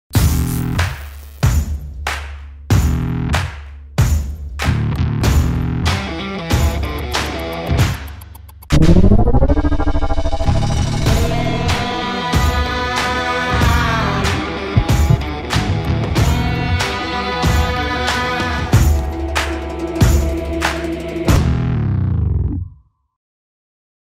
SAP NOW Teaser Video - Powered by Nytro Marketing
The SAP NOW event is one of the newest SAP staples. They asked us to put together this short event teaser, which audiences far and wide loved and engaged with. Are you looking for an experienced company to put together your event teaser reel?